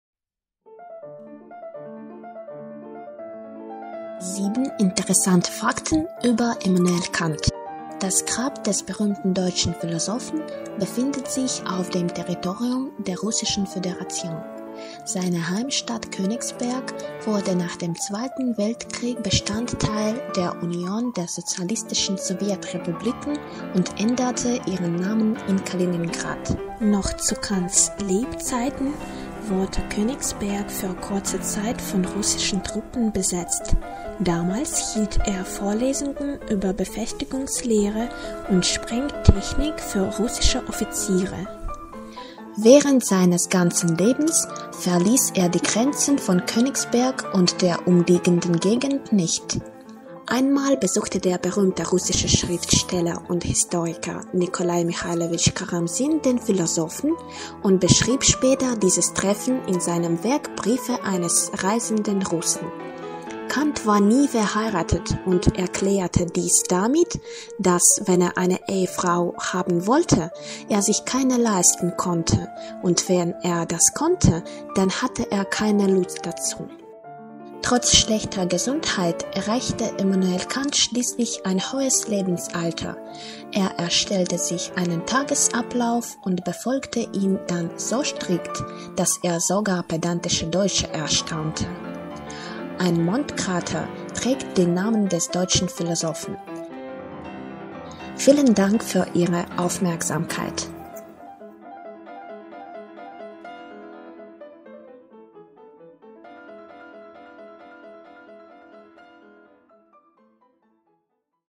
7 interessante Fakten über Kant |7 интересных фактов о Канте (Deutsch B1-C2)
Der Denker Immanuel Kant  (22. April 1724 bis 12. Februar 1804) markiert einen Wendepunkt in der Philosophie: er rief auf,  die Verantwortung für sein eigenes Handeln selbst zu übernehmen, indem man nachdenkt und sein Handeln reflektiert.
Das berühmteste Werk:"Kritik der reinen Vernunft"
Die welweit bekannten Zitate  von ihm:
- Der ziellose Mensch erleidet sein Schicksal, der zielbewusste gestaltet es.
- Habe Mut, dich deines eigenen Verstandes zu bedienen!
- Was kann ich wissen? Was kann ich tun?
- Kein Mensch ist so wichtig, wie er sich nimmt.

Учебный ролик на немецком языке об интересных фактах из жизни Иммануила Канта.
1) Могила знаменитого немецкого философа находится на территории РФ. Кёнисберг – его родной город – после Второй мировой войны вошел в состав СССР и сменил название на Калининград.
Das Grab des berühmten deutschen Philosophen befindet sich auf dem Territorium der Russischen Föderation. Seine Heimatstadt Königsberg wurde nach dem Zweiten Weltkrieg Bestandteil der UdSSR (Union der Sozialistischen Sowjetrepubliken) und  änderte ihren Namen in Kaliningrad.

2)  Eще при жизни Канта Кенисберг был ненадолго захвачен русскими  войсками. Тогда он читал русским офицерам лекции по фортификации и пиротехнике.
Noch  zu Kants Lebzeiten wurde Königsberg für kurze Zeit von russischen Truppen besetzt. Damals hielt er Vorlesungen über Befestigungslehre und Sprengtechnik für russische Offiziere.
3) За всю свою жизнь Кант не покидал пределов Кенисберга и его окрестностей.
Während seines ganzen Lebens verließ er die Grenzen von Königsberg und der umliegenden Gegend nicht.
4) Однажды в гости к философу зашел известный русский писатель и историк  Николай Михайлович Карамзин и описал позднее эту встречу в «Письмах русского путешественника».
Einmal besuchte der berühmte russische Schriftsteller und Historiker Nikolay Mikhailovich  Karamzin den Philosophen und beschrieb später dieses Treffen in seinem Werk  «Briefe eines reisenden Russen».
5) Кант никогда не был женат, объясняя это тем, что когда он хотел иметь жену, он не мог её содержать, а когда уже мог — то не хотел.
Kant war nie verheiratet und erklärte dies damit, dass, wenn er eine Ehefrau haben wollte, er sich keine leisten konnte und wenn er das konnte - dann hatte er keine Lust dazu.
6) Кант прожил долгую жизнь, несмотря на слабое здоровье. Он разработал для себя режим дня и следовал ему неукоснительно, что удивляло даже педантичных немцев.
Trotz schlechter Gesundheit erreichte Immanuel Kant schließlich ein hohes Lebensalter. Er erstellte sich einen Tagesablauf und befolgte ihn dann so strikt, dass es sogar pedantische Deutsche erstaunte.
7) Один из кратеров на Луне носит имя немецкого философа.
Ein Mondkrater trägt den Namen des deutschen Philosophen.

Vielen Dank für Ihre Aufmerksamkeit